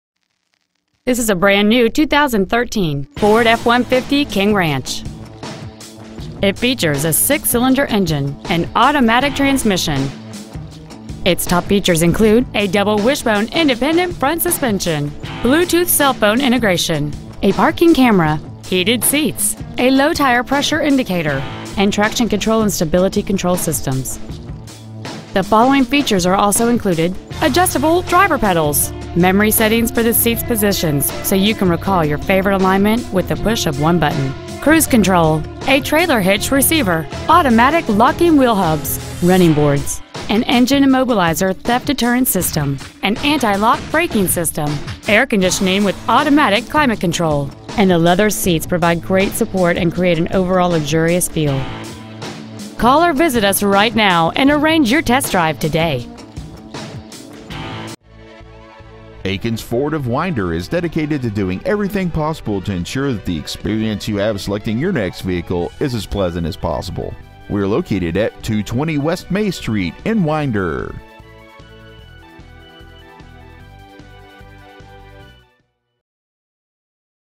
2013 FORD F-150 Winder GA
Year : 2013
 Make : FORD
 Model : F-150
 Engine : 3.5L V6 24V GDI DOHC TWIN TURBO
 Trans . : 6-SPEED AUTOMATIC
 Exterior : WHITE PLATINUM METALLIC TRI-COAT
 Miles : 6

 Stock : DKE31925